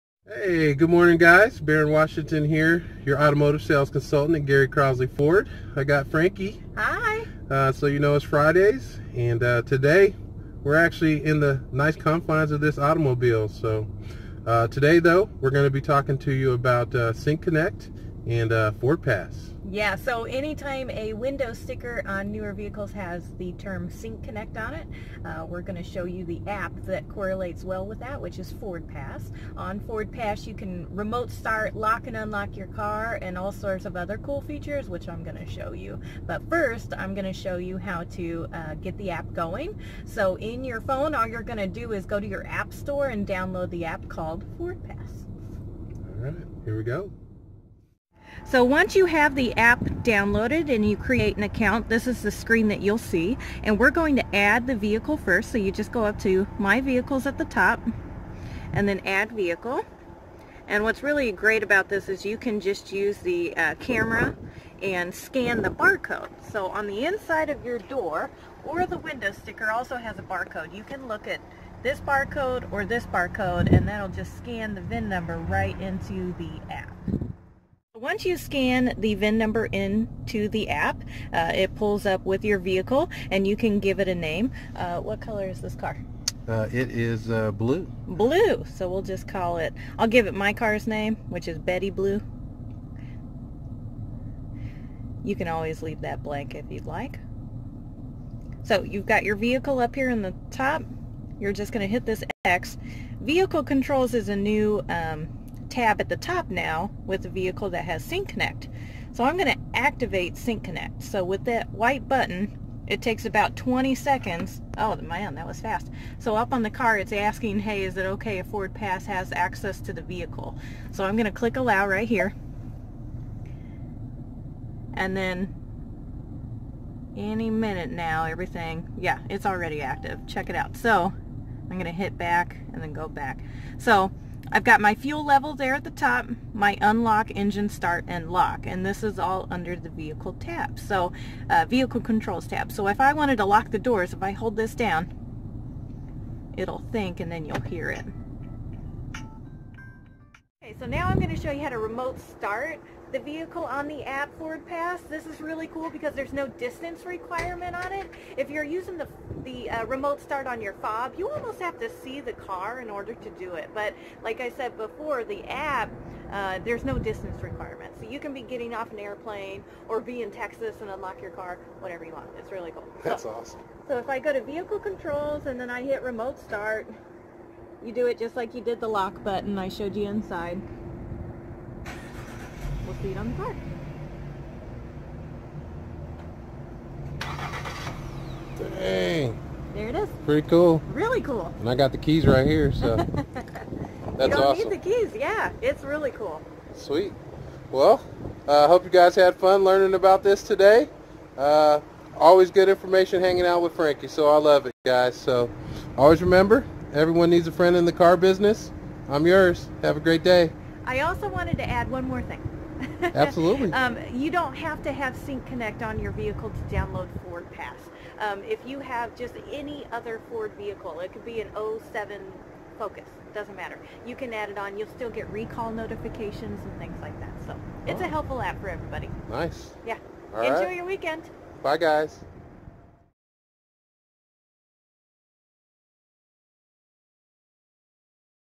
Lock, unlock, and start your vehicle from anywhere in the world!!
Sync Connect and Ford pass allows you so many wonderful options!!